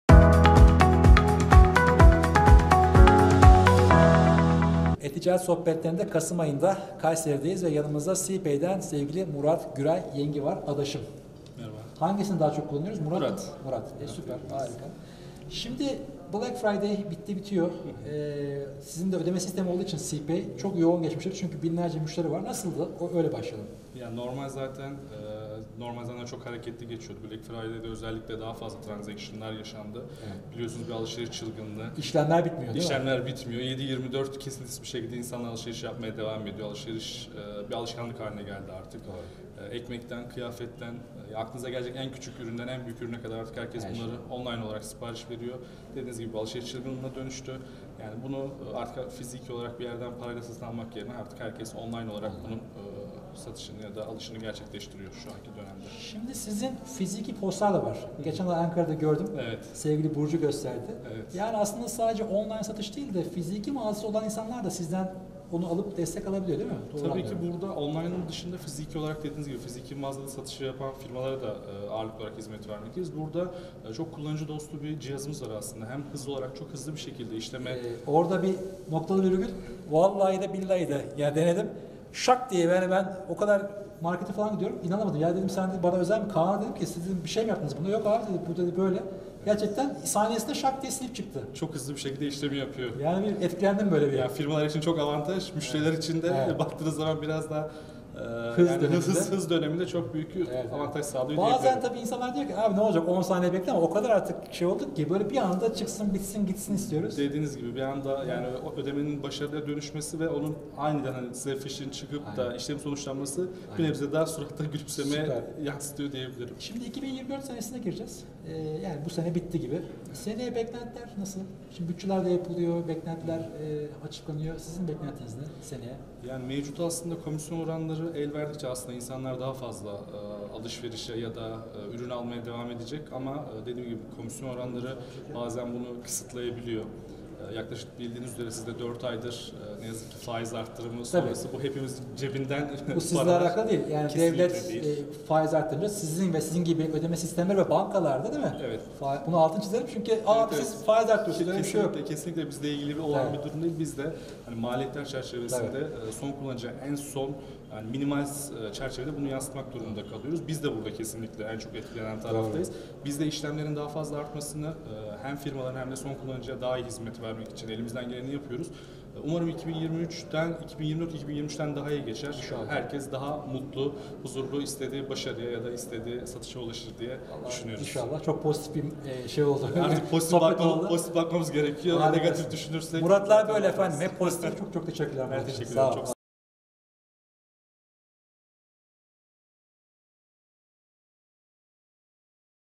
Black Friday Dönemi Oldukça Hareketli Geçti - Murat Güray Yengi (Sipay)
Kayseri'de düzenlediğimiz E-Ticaret Sohbetleri Anadolu adlı program öncesinde Sipay Bölge Satış Yöneticisi Murat Güray Yengi ile yaptığımız röportaja buradan bakabilirsiniz.